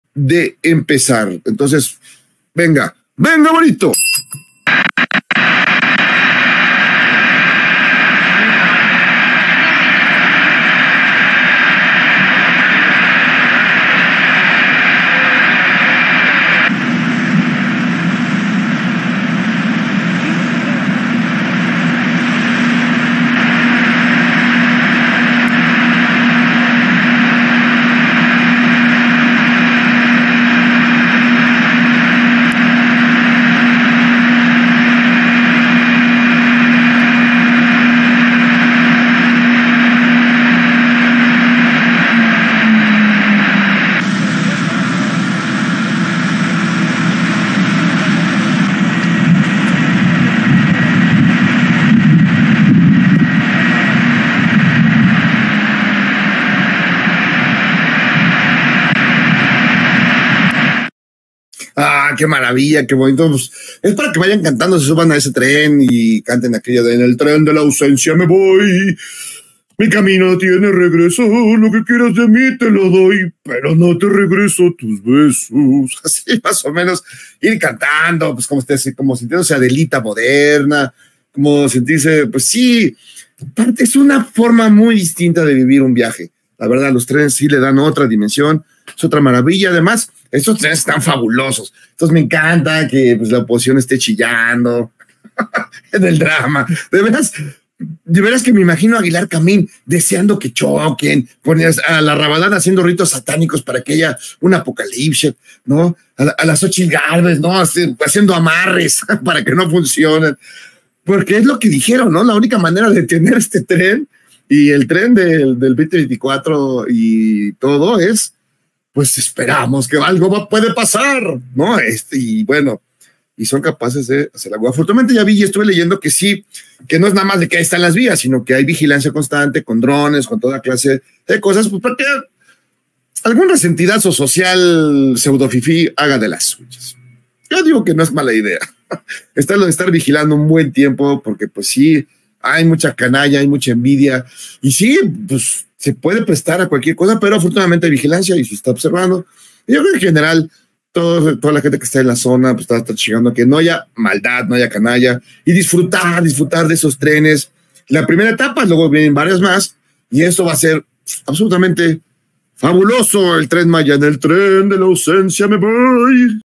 SE INAUGURA EL TRENMAYA Y LA OPOSICIÓN SIGUE SIN CREERLO
Jairo Calixto hace presente la inauguración del TrenMaya pues la califica como una nueva experiencia más segura y expone cómo está la oposición ante este hecho, ya que no creían posible que el presidente del pueblo, Andrés Manuel López Obrador pudiera verlo terminado antes de finalizar su mandato.